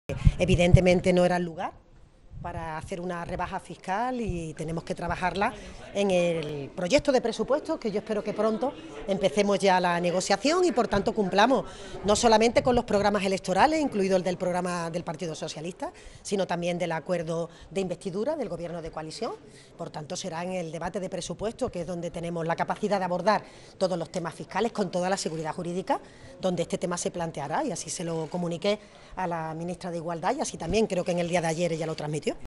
Montero vincula la bajada del IVA de higiene femenina al proyecto de PGE
La ministra de Hacienda y Función Pública, María Jesús Montero, ha asegurado que esta ley "no era el lugar" para impulsar esta rebaja en el IVA aplicado a los productos de higiene femenina, subrayando que "no hay una discusión sobre el fondo", sino que "es un elemento de oportunidad y de seguridad jurídica".

(Fuente: Congreso)